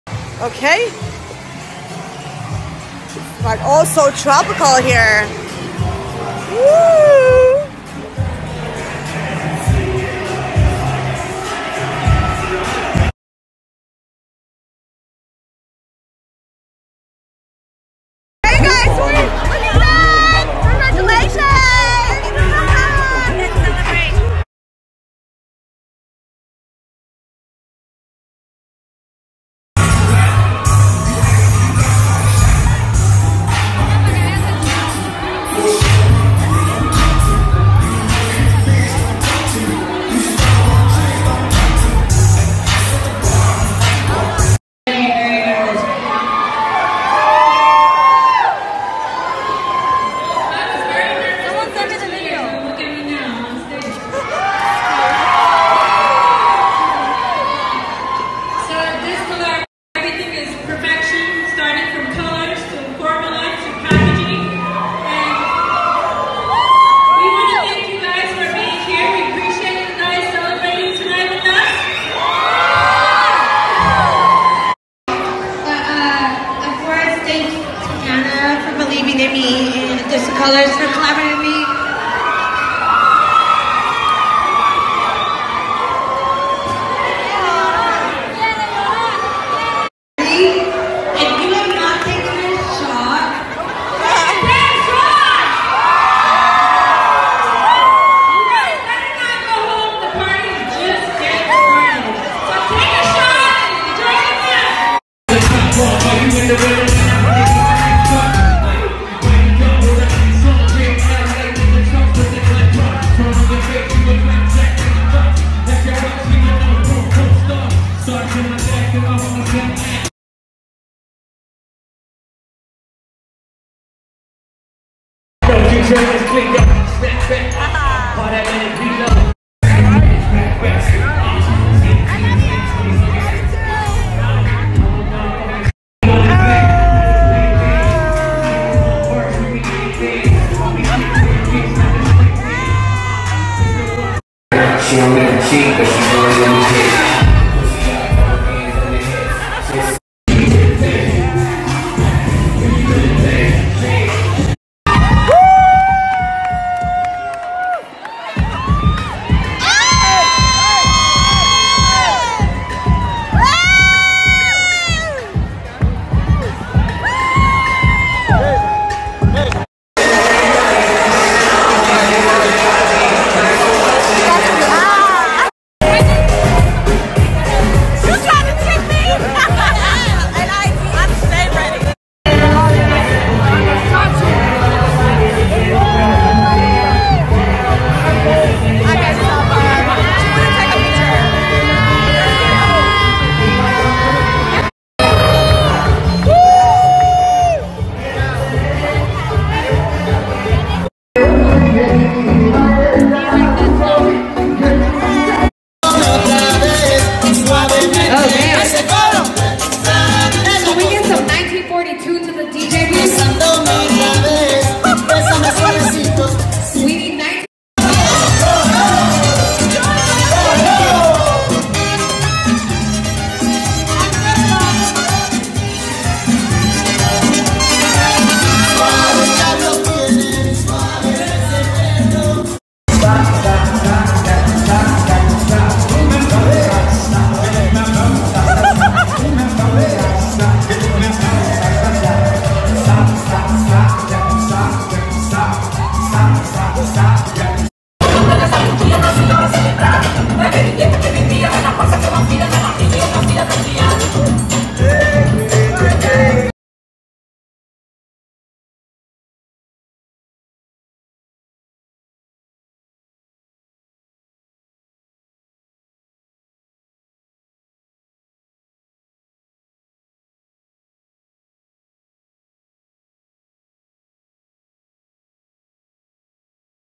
LAUNCH EVENT I LUV SARAH ii X DOSE OF COLORS
The whole collection 🚨 NEW Collaboration @iluvsarahii X @doseofcolors
Includes:
1. EyeshadowPalette $35
2. 2 Highlighters $28 Each
Sol Mate - gold
Bathe - peach pink
3. 4 Matte LiquidLipstick $18 Each
Nude Chica - pinky nude
Peachii - peachy nude
Fresa - bright strawberry
Aesthetic - burnt sienna
4. 2 Glosses $17 Each:
Barely There - pink nude, sheer
Brillo - gold reflects .

Will be available➡️ JULY 27TH @ 10am PST

VIDEO CREDIT : TRENDMOOD1